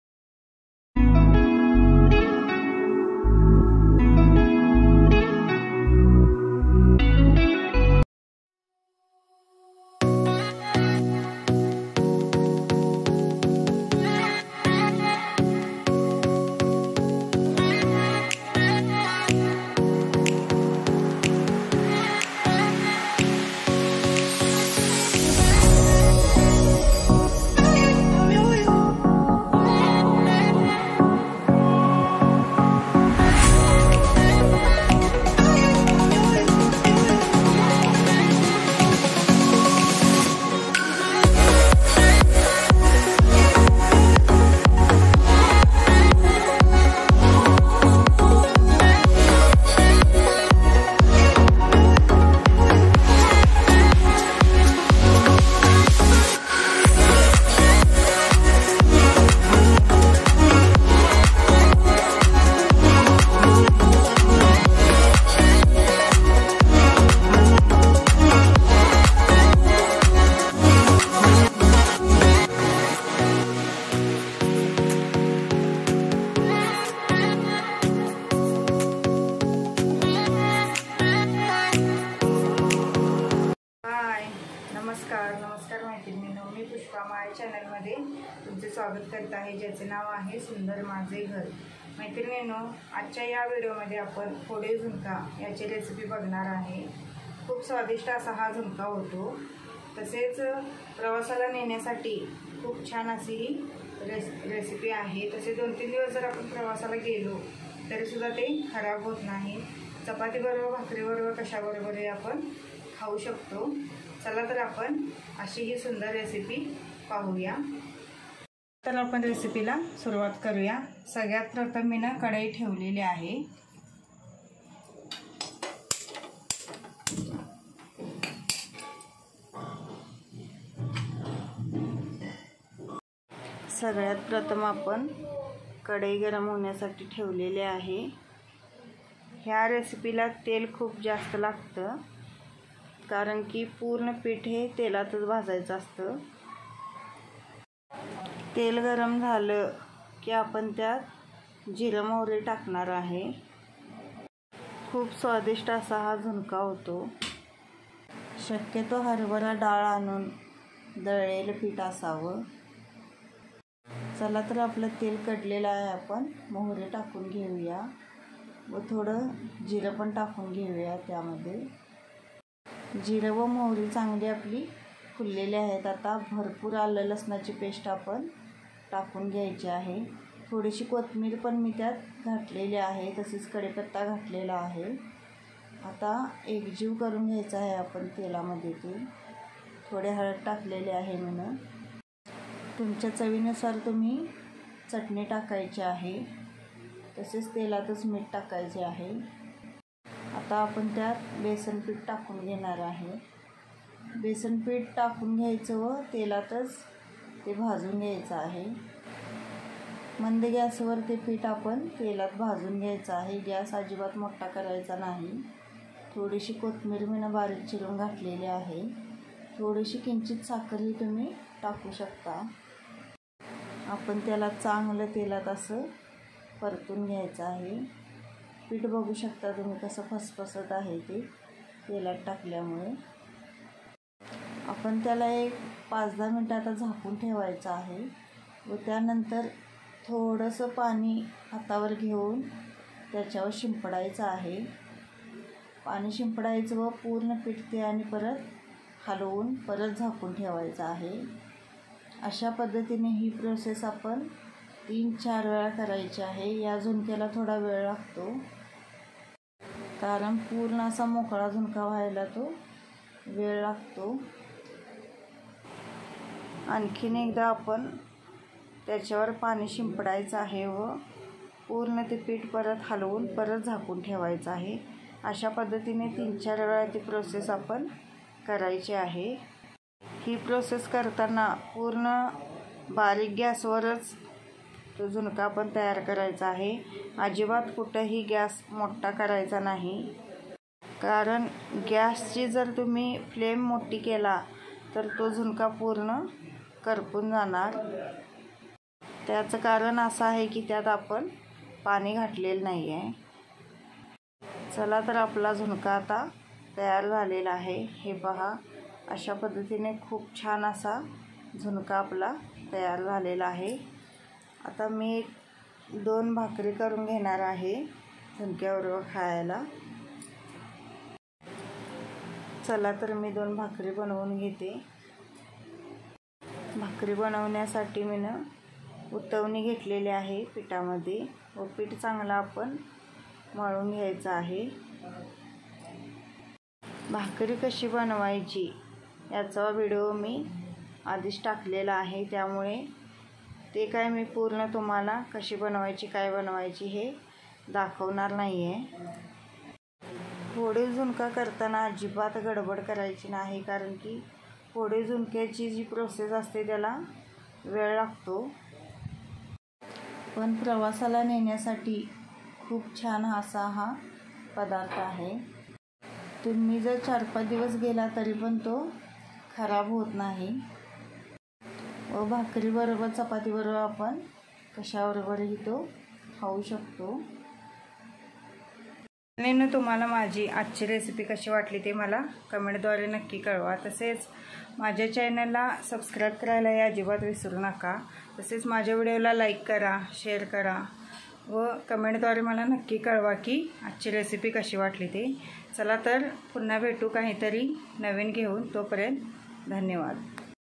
#फोडीव झुणका😋😋//🙏🙏 सुट्टा झुणका रेसिपी ## भाकरी व चपाती 🍛🍛 कश्या बरोबर ही ❤️❤️ खाऊ शकतो😍😍
मैत्री निनो मी आज या व्हिडिओ मध्ये तुम्हाला मोकळा झुणका भाकरी🧆🧆🧆याची रेसिपी दाखवणार आहे ती तुम्हाला नक्किच आवडेल🙏🙏🙏 धन्यवाद